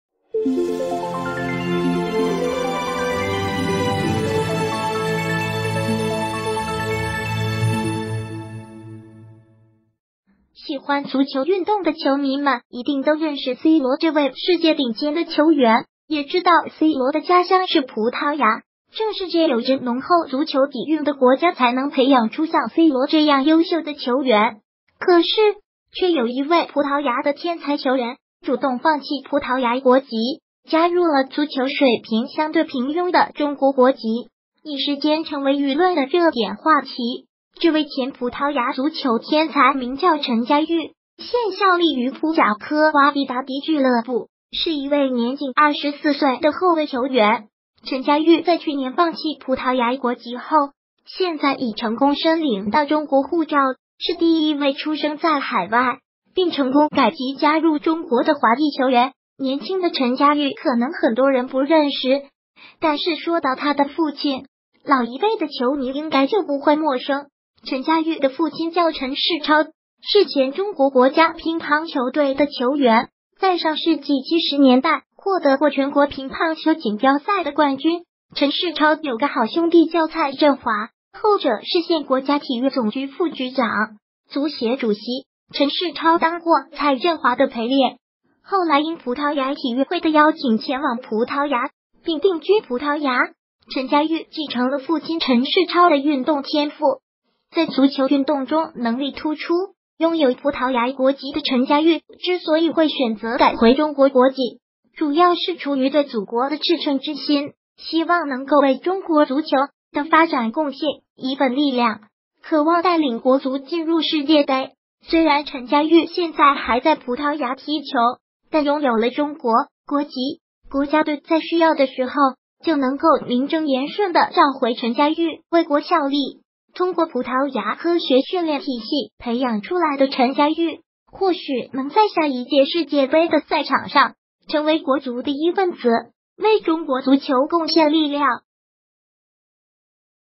赤子之心 葡萄牙天才入籍中国 渴望带领国足进入世界杯 为国效力
喜欢足球运动的球迷们一定都认识C罗这位世界顶尖的球员，也知道C罗的家乡是葡萄牙，正是这有着浓厚足球底蕴的国家才能培养出像C罗这样优秀的球员。可是，却有一位葡萄牙的天才球员主动放弃葡萄牙国籍，加入了足球水平相对平庸的中国国籍，一时间成为舆论的热点话题。    这位前葡萄牙足球天才名叫陈佳裕，现效力于葡甲科瓦彼达迪俱乐部，是一位年仅24岁的后卫球员。陈佳裕在去年放弃葡萄牙国籍后，现在已成功申领到中国护照，是第一位出生在海外，并成功改籍加入中国的华裔球员。    年轻的陈佳裕可能很多人不认识，但是说到他的父亲，老一辈的球迷应该就不会陌生。陈佳裕的父亲叫陈仕超，是前中国国家乒乓球队的球员，在上世纪70年代获得过全国乒乓球锦标赛的冠军。陈仕超有个好兄弟叫蔡振华，后者是现国家体育总局副局长，足协主席。    陈仕超当过蔡振华的陪练，后来应葡萄牙体育会的邀请前往葡萄牙，并定居葡萄牙。陈佳裕继承了父亲陈仕超的运动天赋，在足球运动中能力突出。拥有葡萄牙国籍的陈佳裕之所以会选择改回中国国籍，主要是出于对祖国的赤诚之心，希望能够为中国足球的发展贡献一份力量，渴望带领国足进入世界杯。    虽然陈佳裕现在还在